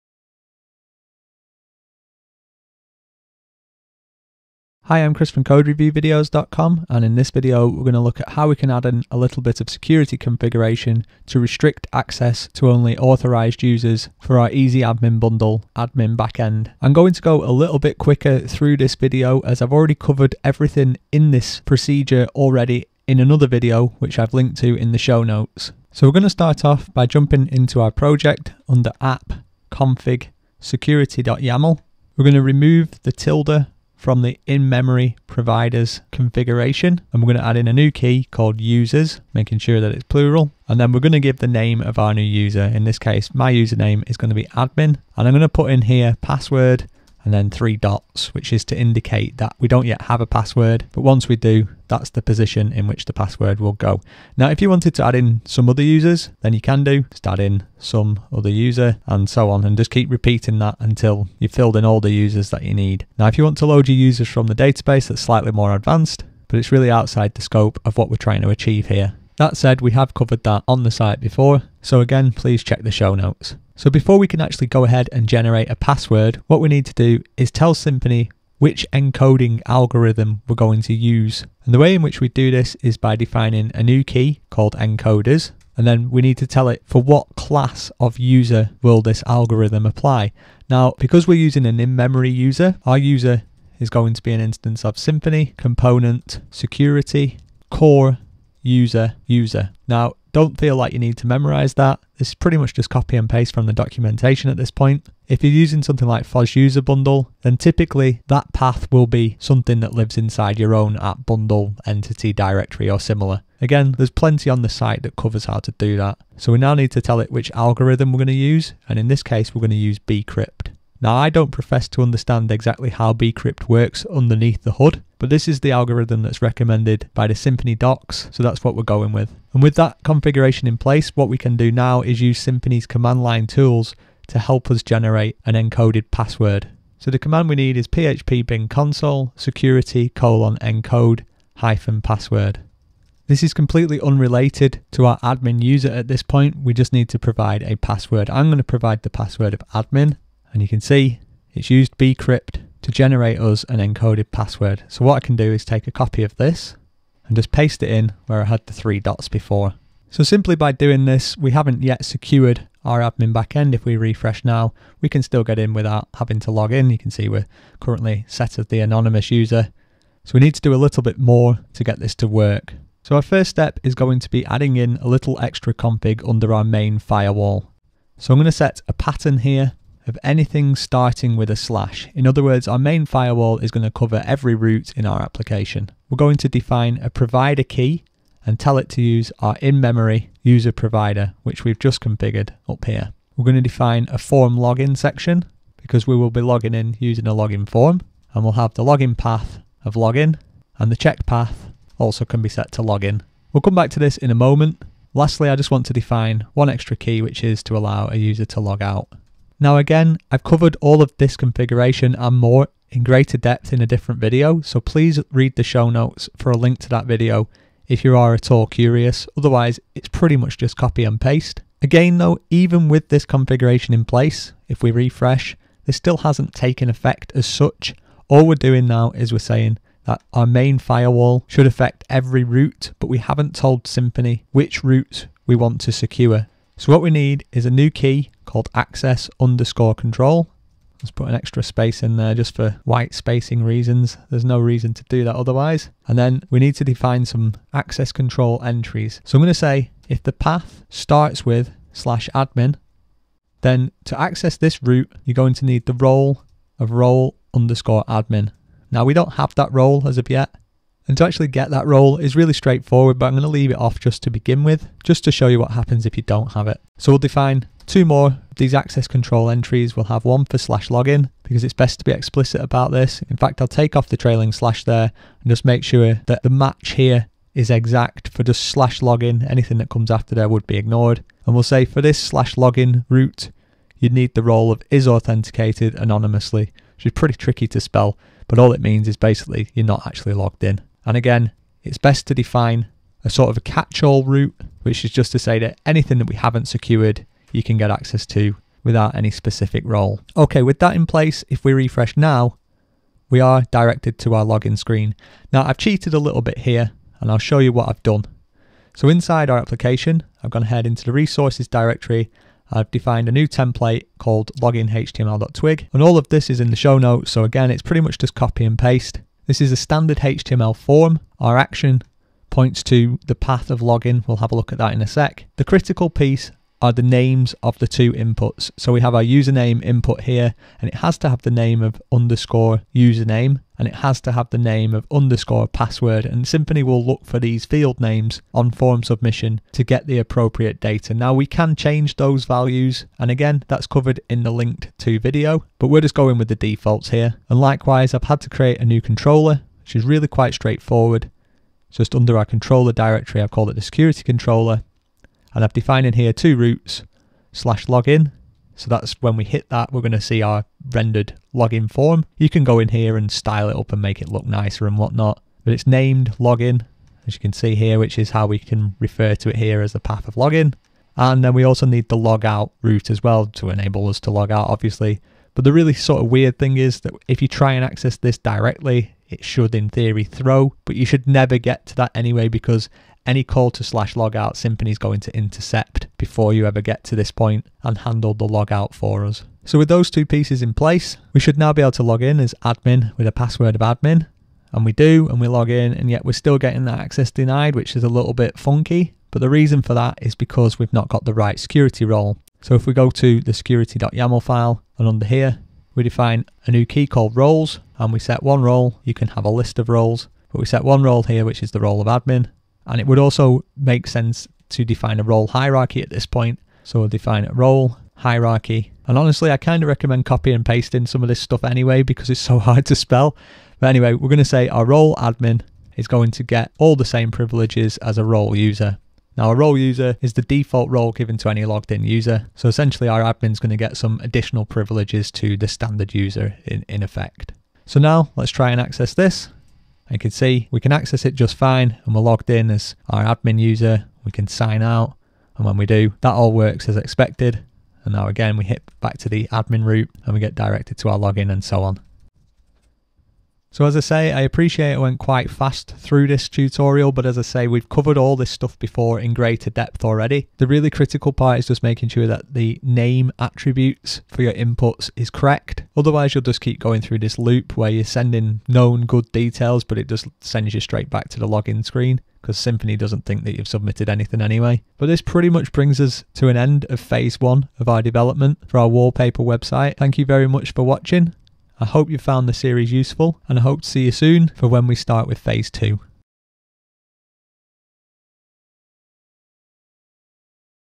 [38/38] - EasyAdminBundle Login Form Tutorial
Learn how to quickly and easily add a secured Login Form to your Symfony 3 EasyAdminBundle admin back end setup with this free tutorial